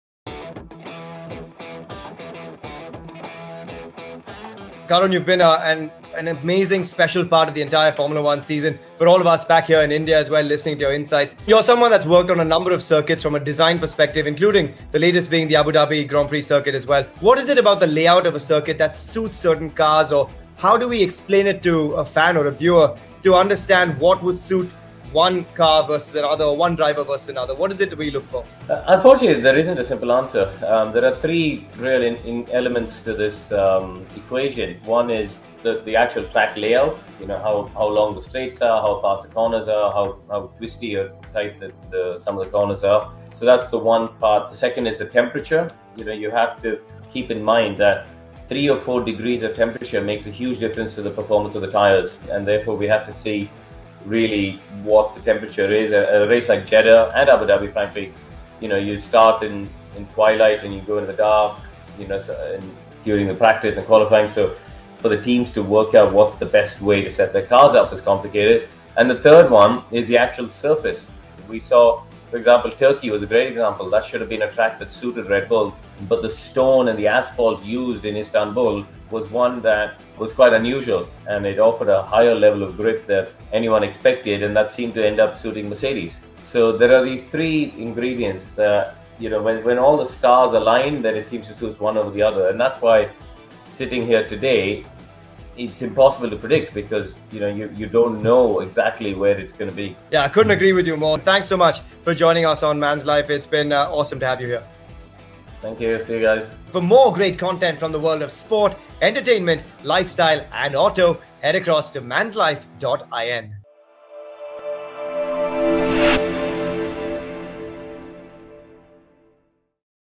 F1 circuits with Karun Chandhok| Formula One Racing| F1 Race
A multi-race winner and podium runner in GP2, Karun Chandhok was a Red Bull Racing Formula One team test driver in 2007 and 2008, then went on to drive Formula One in 2010 and 2011, respectively, for both Hispania Racing and Team Lotus. Since 2016, Karun Chandhok has been an occasional driver and advisor to Williams Formula One Heritage activities, working with the Williams Formula One Team in expanding that side of its business, while also showing off its World Formula One. Karun Chandhok has also joined Mahindra Racing in the brand-new Formula E Series, the new historical Championship for electric racing cars, in its inaugural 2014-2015 season, having agreed his entry to the Series. Karun Chandhok has also worked with Nissan on running a GT India, the Tata Group on T1 truck racing series, with JK Tyres on various Formula E Holdings and the Mahindra Group in the Series.